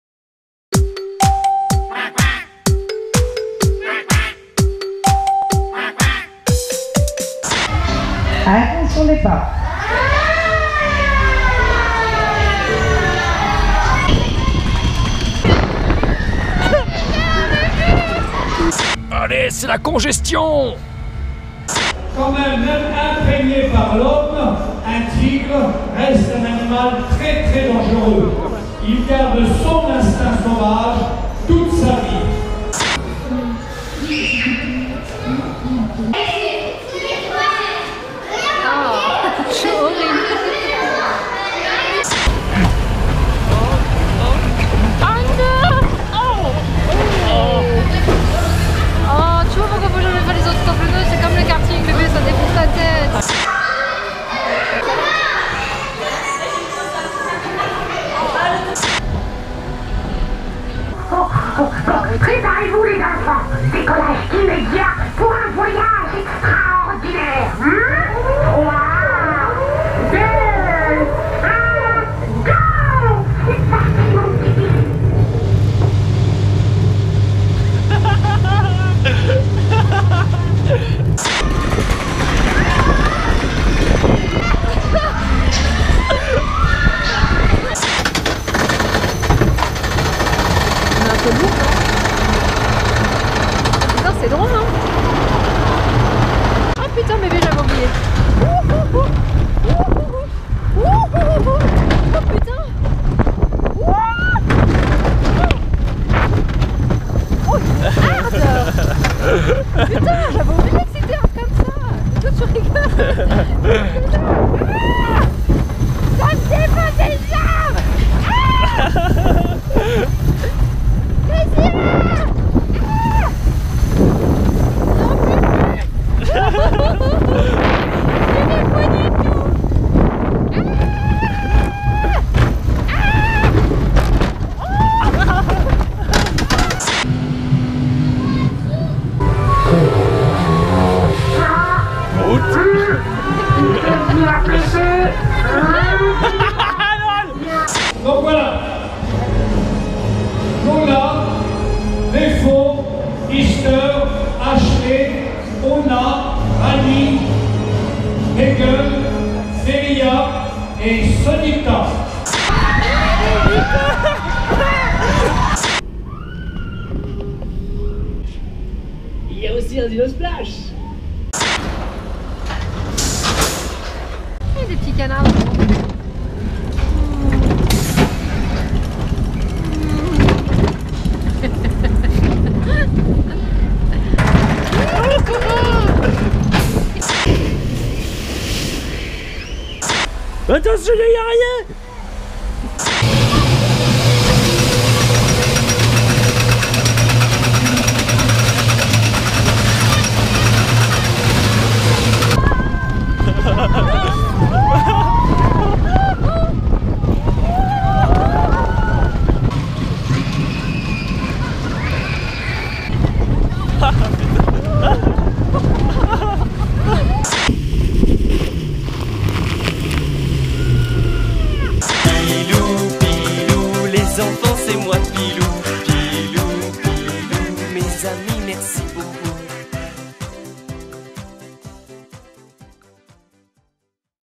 #59 - ZAPPING au Parc Saint Paul - GoPro 4
Dernière vidéo de l'année avec le Parc Saint Paul cette fois-ci. Elle nous y a emmené le jeudi 22 octobre 2015, et ce fût une agréable journée de plus avec beaucoup de rires.

Nous vous donnons rendez-vous à l'année prochaine, en janvier 2016, pour de nouvelles vidéos et aventures !

Filmé avec GoPro 4 silver
Stabilisé avec Feiyu Tech G3 Ultra